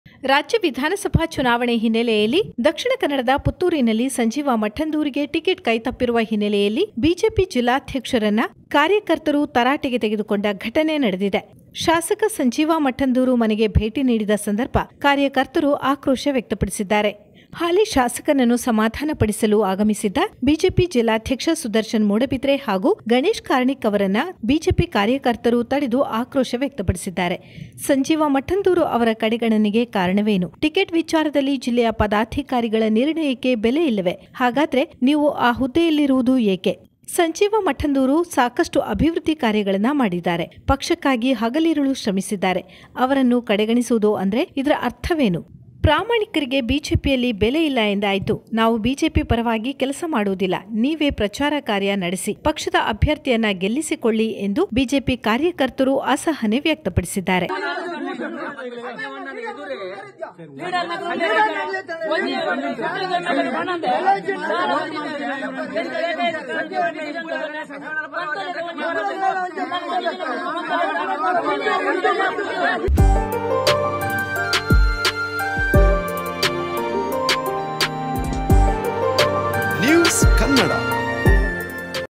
ಪುತ್ತೂರು: ಮಠಂದೂರಿಗೆ ಟಿಕೆಟ್ ಮಿಸ್ : ಸಮಾಧಾನಿಸಲು ಹೋದ ಜಿಲ್ಲಾಧ್ಯಕ್ಷನ ವಿರುದ್ಧ ಬೆಂಬಲಿಗರು ಗರಂ
ಪುತ್ತೂರಿನಲ್ಲಿ ಸಂಜೀವ ಮಠಂದೂರು ಗೆ ಟಿಕೆಟ್ ಕೈ ತಪ್ಪಿದ ಹಿನ್ನಲೆ ಹಾಲಿ ಶಾಸಕನನ್ನ ಸಮಾಧಾನಿಸಲು ಆಗಮಿಸಿದ್ದ ಜಿಲ್ಲಾಧ್ಯಕ್ಷ ಸುದರ್ಶನ್ ಮೂಡಬಿದಿರೆ ಹಾಗೂ ಗಣೇಶ್ ಕಾರ್ಣಿಕ್ ರನ್ನ ತಡೆದು ಕಾರ್ಯಕರ್ತರು ತಮ್ಮ ಆಕ್ರೋಶವನ್ನು ವ್ಯಕ್ತಪಡಿಸಿದ್ದಾರೆ.